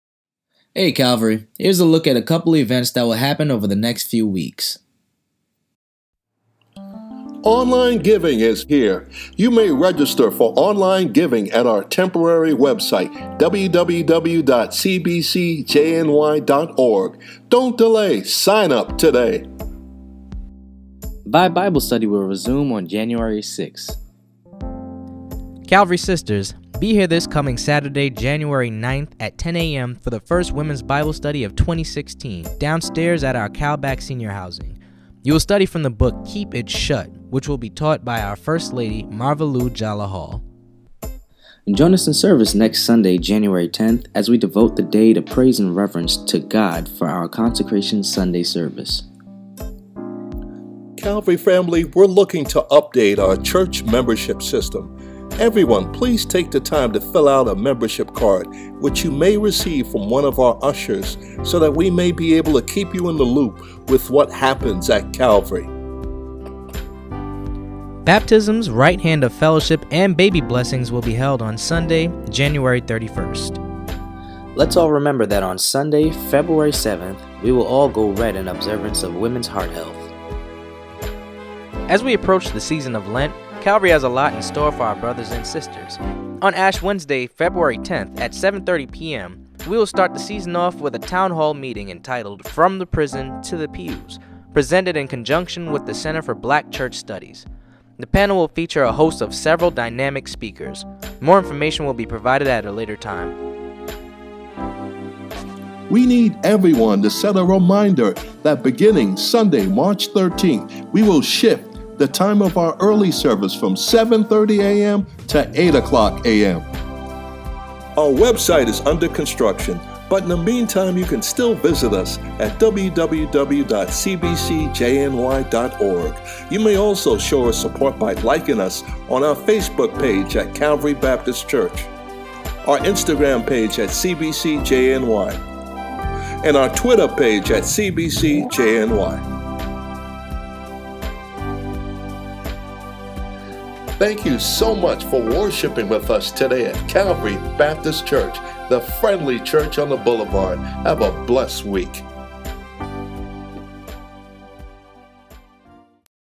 Calvary Church Connections: January 5, 2016
Here is a run down of all events that will take place at Calvary Baptist Church over the next few weeks.